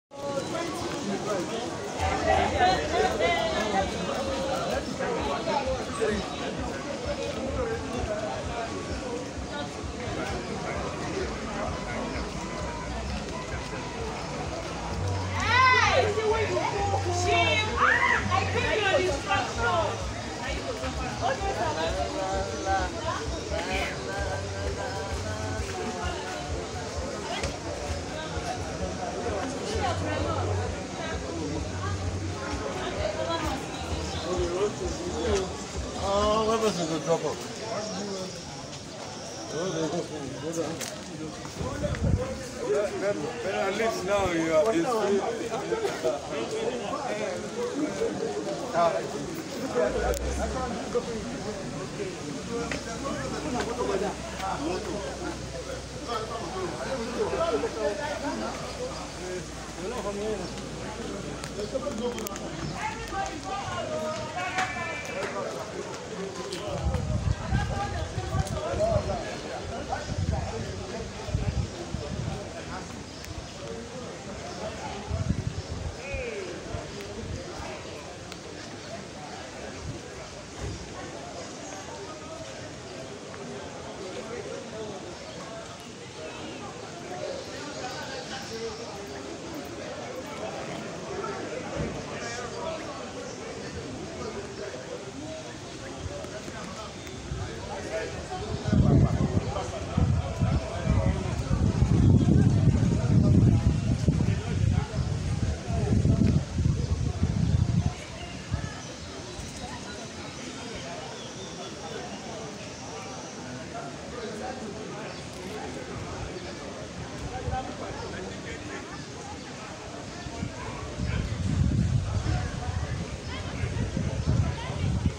This Vehicle Was Used During Nigeria's Independence in 1960. By Who?
A visit to the museum in Jos town, Plateau State revealed this car amongst other historical items showcased at the museum. The Queen of England, Queen Elizabeth 11 is said to have ridden in this vehicle to loud ovation during one of her visits to Nigeria. If you didn't see it then, you have the opportunity now. Answer the question & get a fantastic price for your troubles.